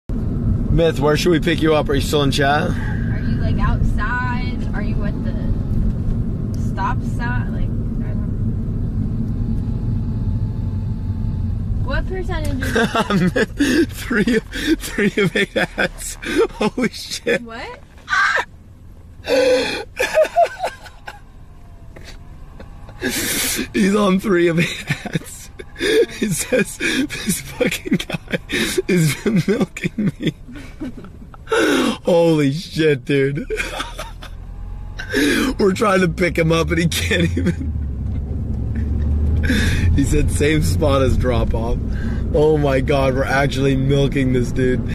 Myth Asked for Blau to Pick Him Up in Chat but He's Stuck Watching 3 of 8 Ads
▼ (￣▽￣)ノ Open me!! ▼